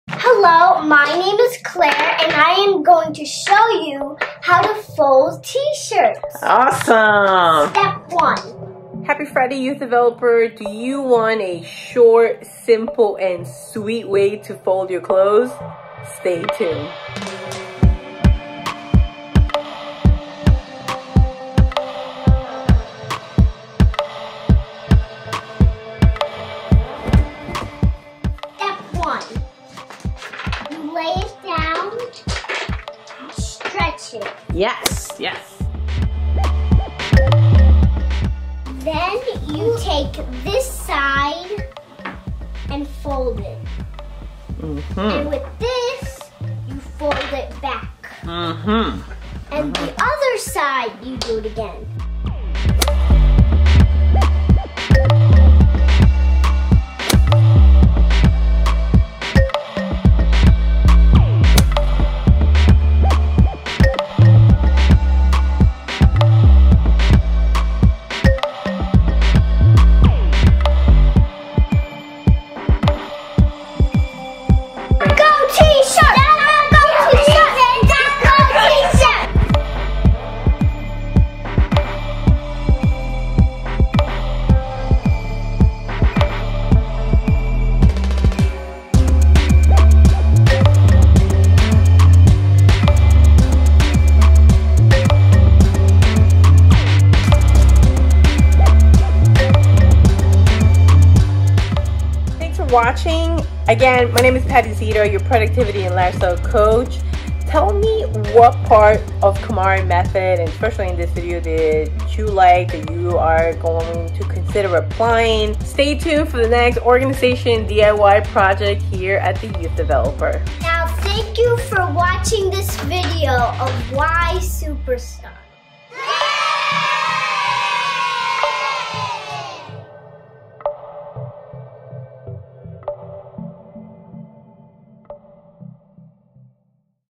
How to KonMari fold a T-shirt
Want a short, simple, and sweet way to fold your t-shirts? Yes, even children can learn how to do it. Grab a t-shirt and let's get started.

What do you like about the KonMari method? Drop it in the comments below.